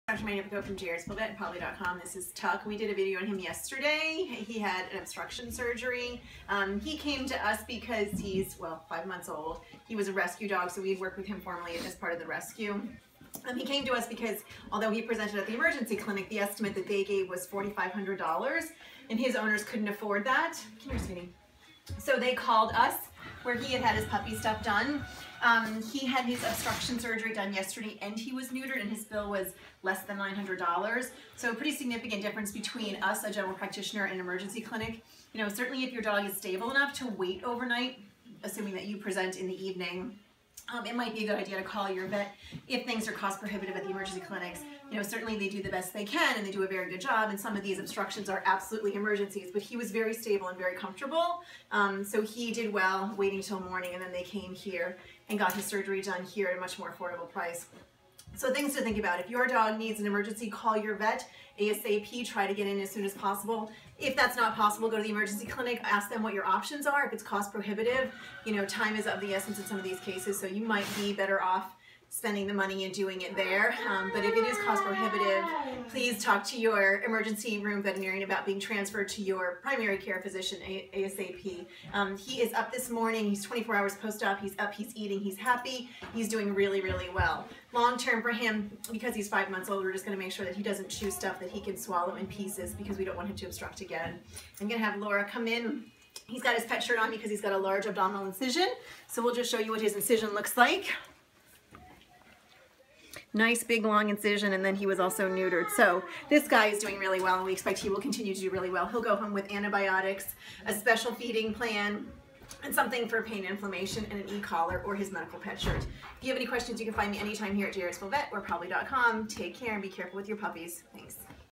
Post operative obstruction surgery in a dog.
Small animal veterinarian Dr Krista Magnifico discusses what options are available for a pet who needs an intestinal obstruction surgery. Cost and options as well as post operative care.